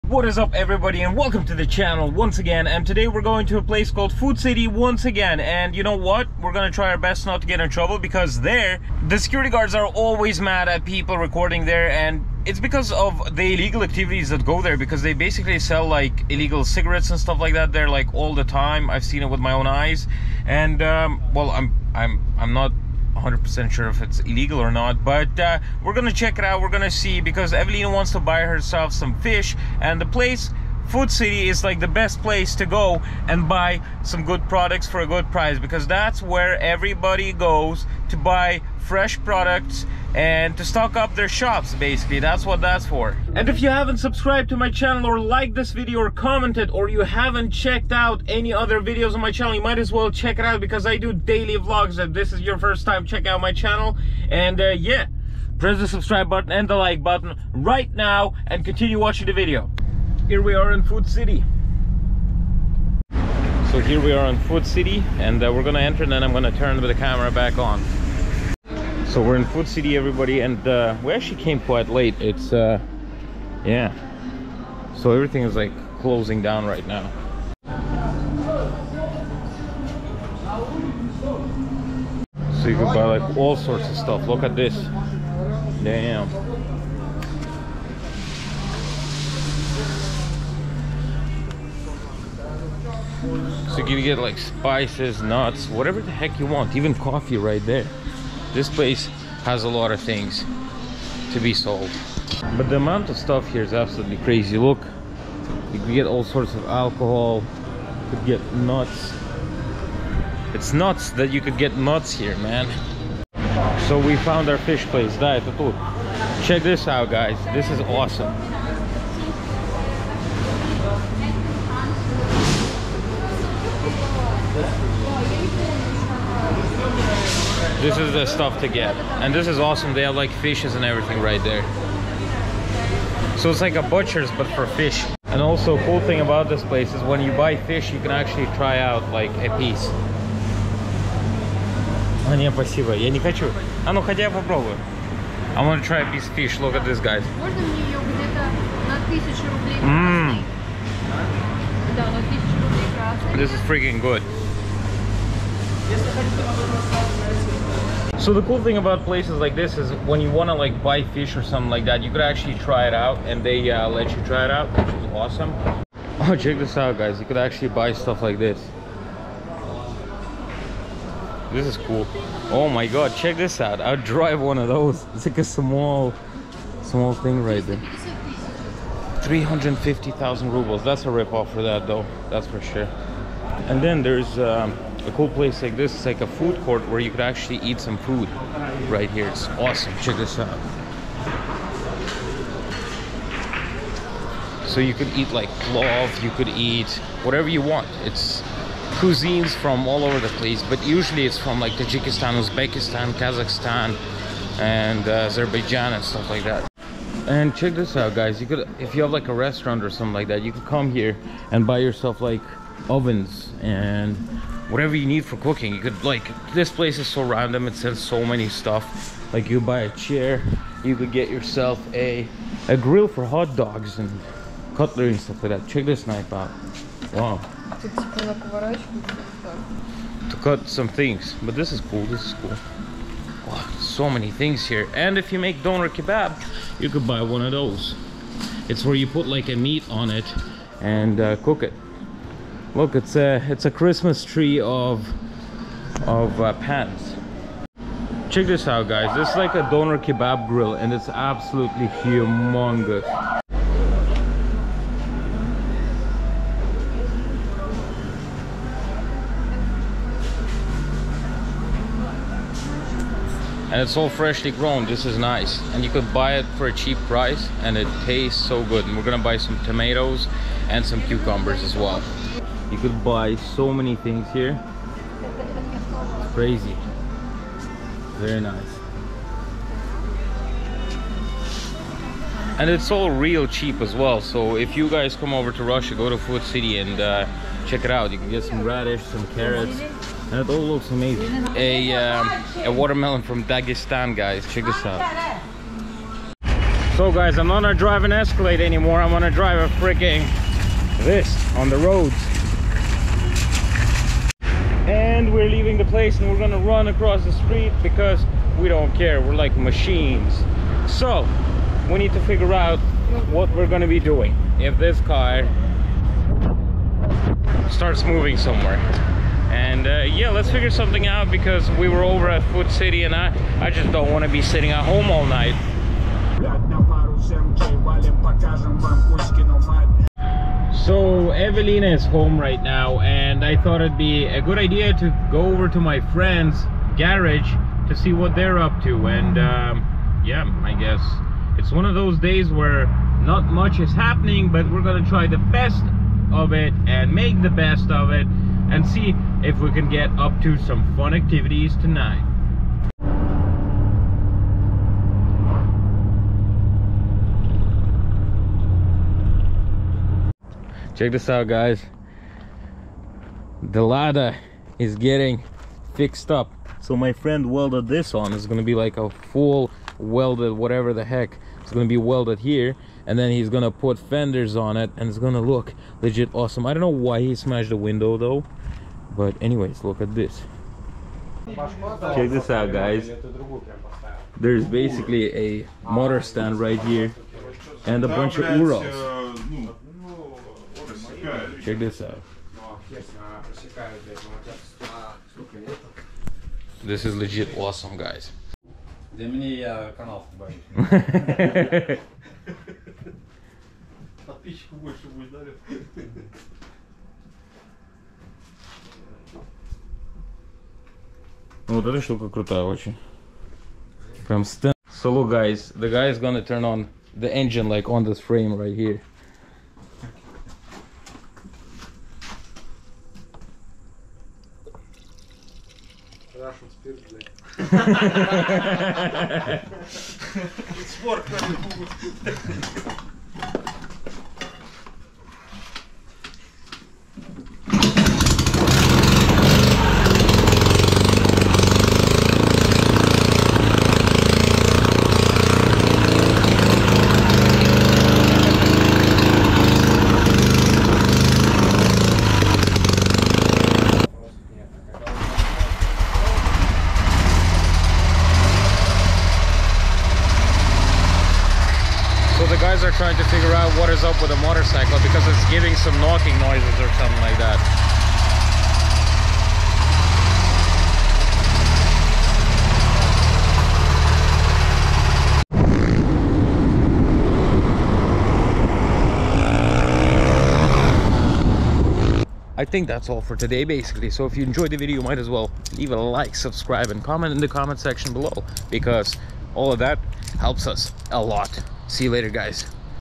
Sanctioned Fish In RUSSIA
This is just a casual day of doing some grocery shopping in Russia during sanctions. Getting a bit of fish and Dagestan Watermelons!